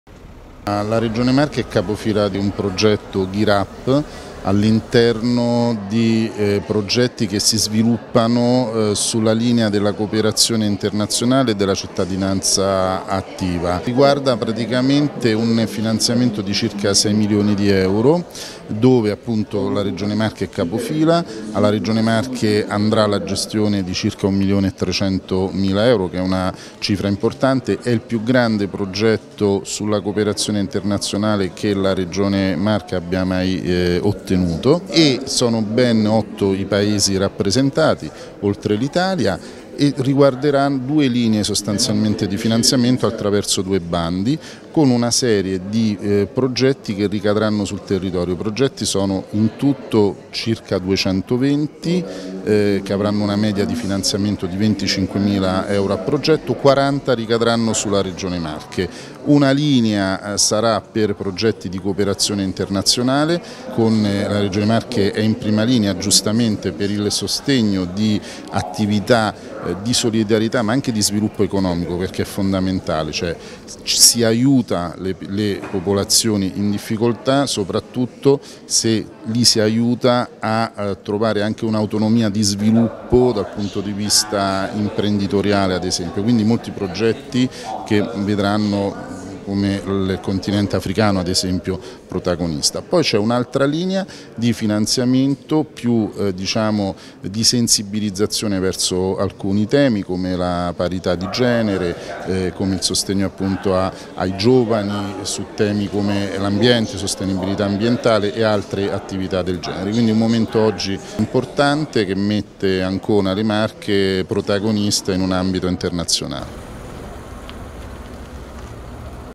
Educazione alla cittadinanza globale: la Regione presenta GEAR UP!
Antonini: “In arrivo 6 milioni di euro per attività di formazione e bandi rivolti ad enti locali ed associazioni”